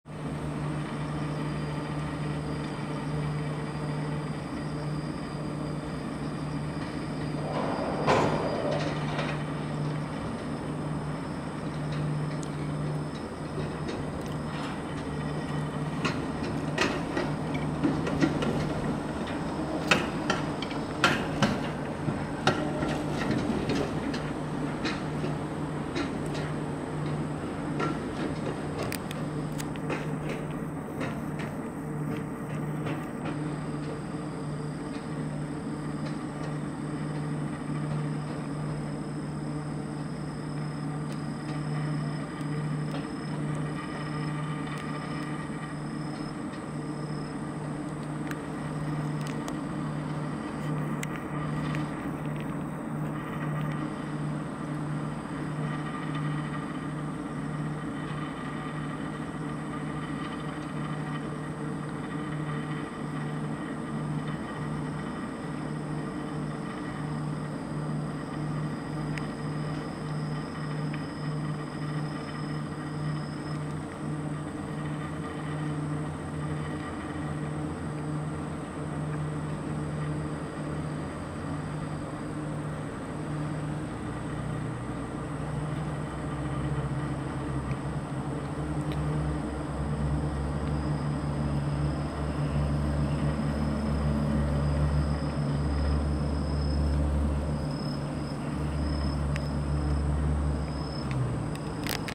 みどり中街駅からみどり中央駅へ向かうスカイレール みどり中街駅付近にて
みどり中街駅を発車するみどり中央駅行きのスカイレールをみどり中街駅付近にて撮影しました。